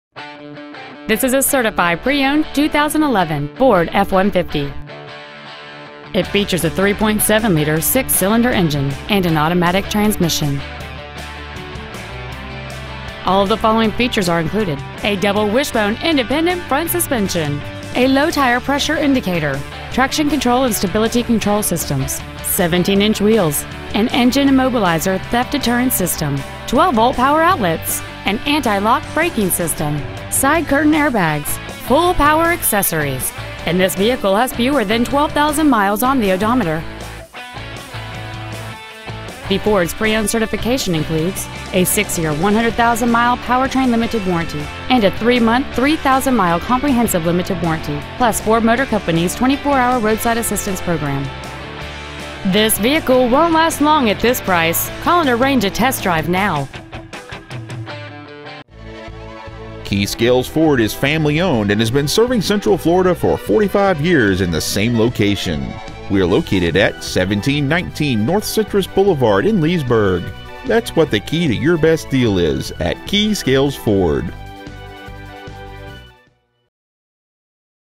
2011 FORD F-150 Certified Leesburg FL
We have been honored to serve the Leesburg FL area , we promise that your experience at our dealership will exceed your expectations !

 Year : 2011
 Make : FORD
 Model : F-150
 Engine : 3.7L V6 24V MPFI DOHC FLEXIBLE FUEL
 Trans . : 6-SPEED AUTOMATIC
 Exterior : STERLING GRAY METALLIC
 Miles : 11,588
 Interior : STEEL GRAY
 Stock : PD13908